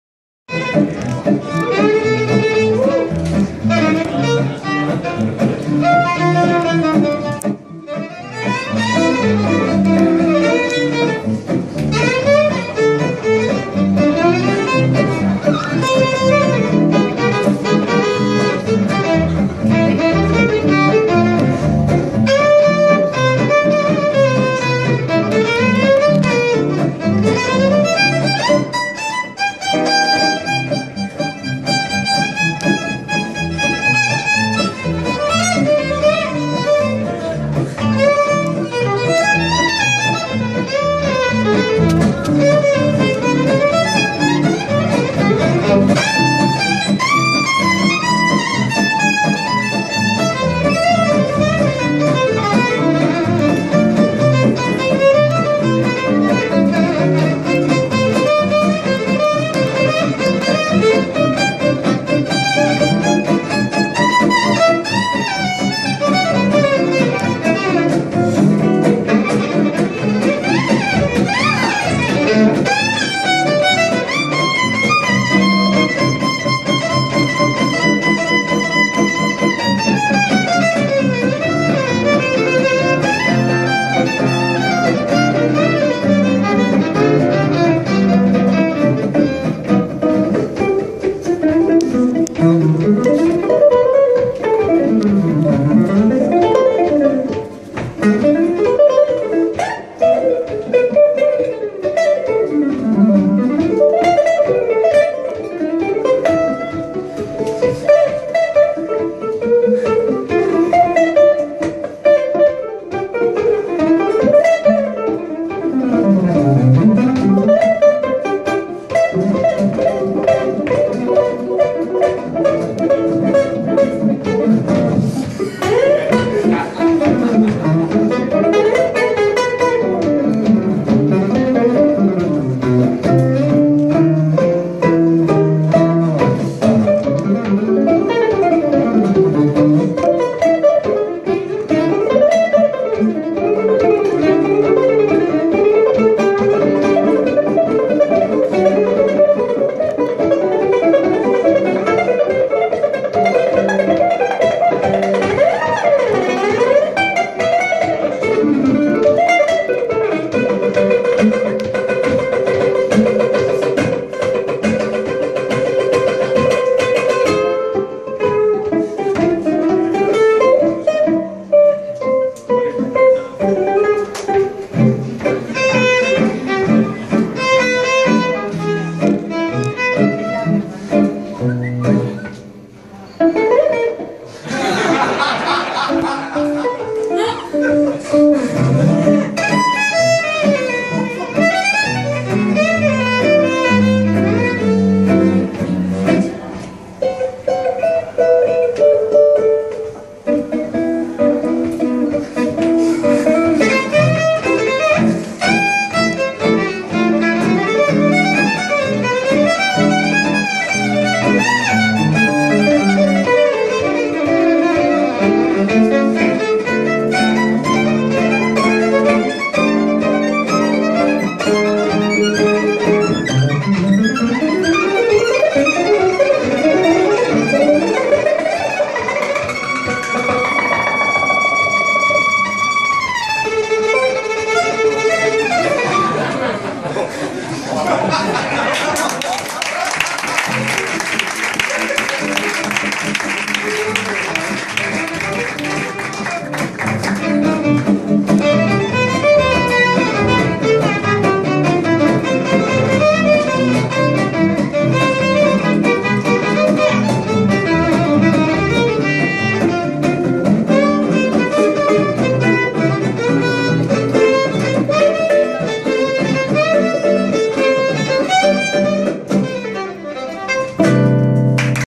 Carbon Fiber Violin Qarbonia Oriol Saña & Albert Bello 1st FIDDLE WEEKEND Madrid hd720
Oriol Saña (playing in a Qarbonia carbon fiber violin) & Albert Bello (guitar) at a concert during the 1st QARBONIA FIDDLE WEEKEND, Madrid (Spain), May 2014.